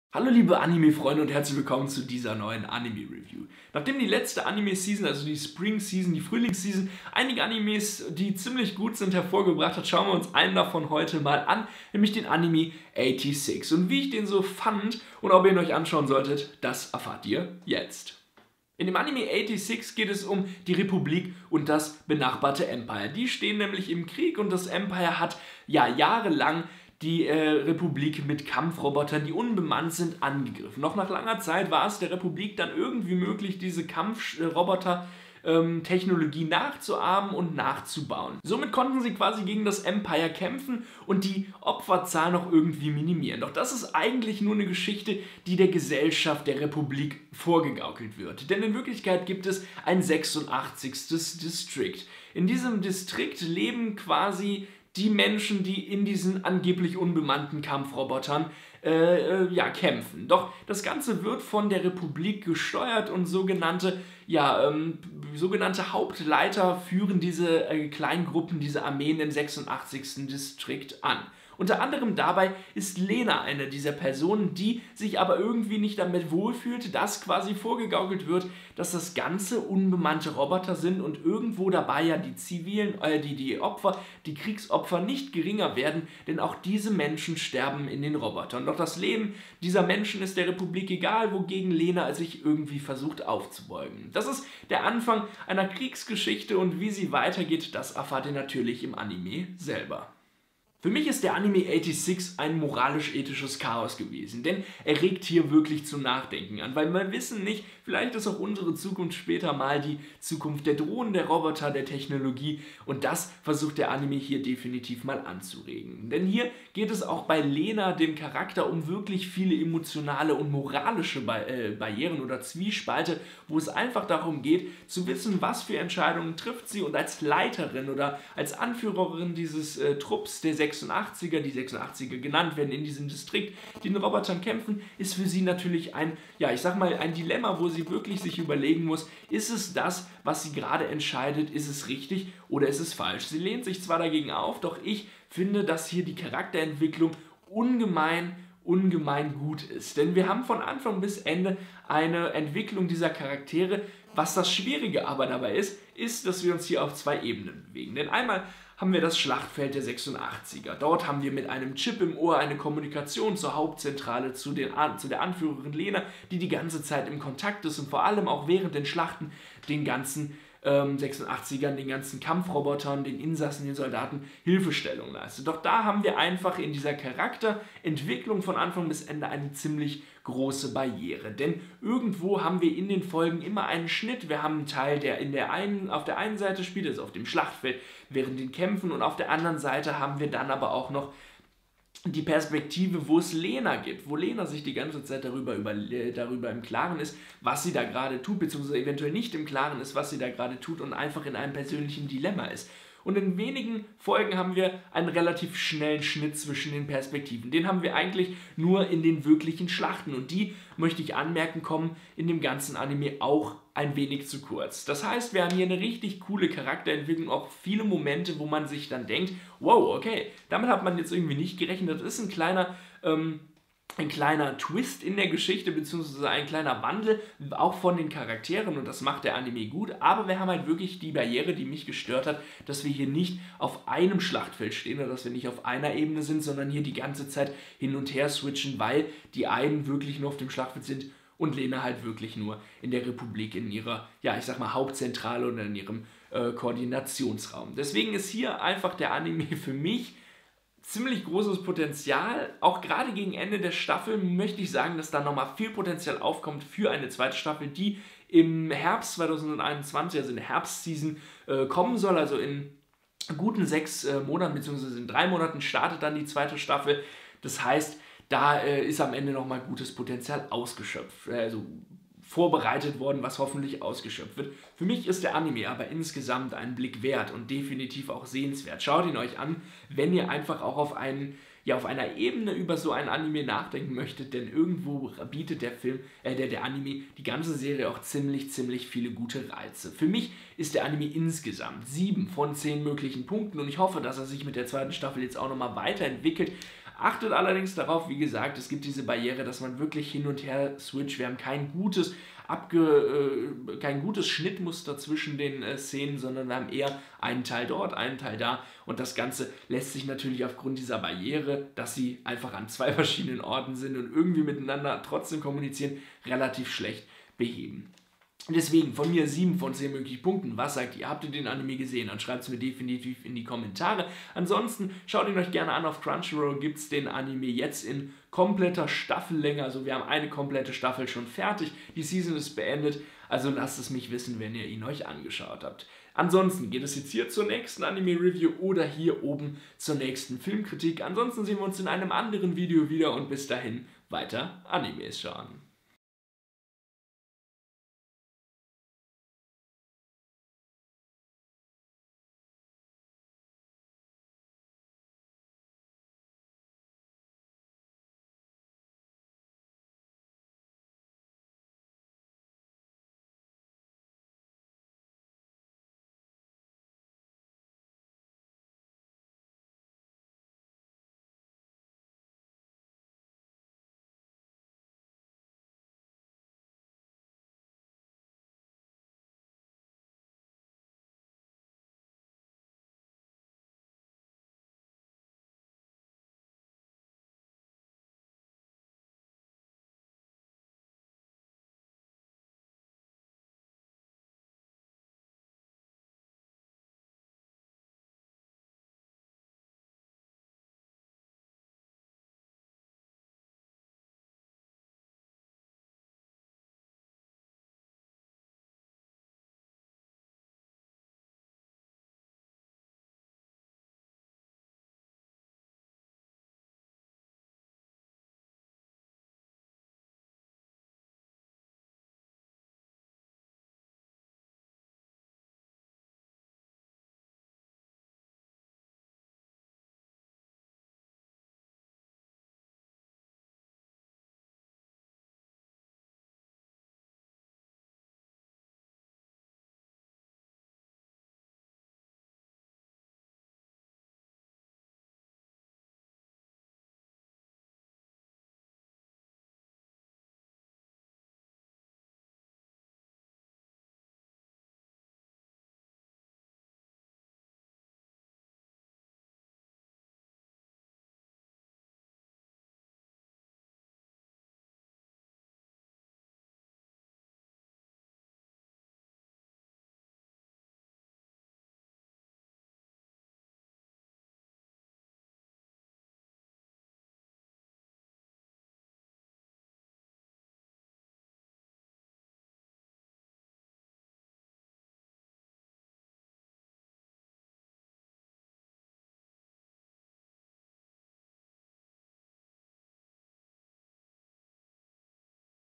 86: EIGHTY-SIX Anime Kritik (2021)
Inhalt: Die Republik von San Magnolia wird seit Langem von einer Armee unbemannter Drohnen des benachbarten Imperiums Giad angegriffen. Nach Jahren mühsamer Forschung hat die Republik schließlich eigene autonome Drohnen entwickelt und den einseitigen Kampf in einen Krieg ohne Opfer verwandelt – zumindest wird das behauptet. In Wahrheit liegt jenseits der Mauern der 85 Zonen ein „nicht existenter“ 86. Sektor. Die jungen Männer und Frauen dieses verlassenen Landes werden als die „Eighty Six“ gebrandmarkt, ihrer Menschlichkeit beraubt und müssen hinter dem Steuer dieser Drohnen um ihr Leben kämpfen … (Quelle: TMDb)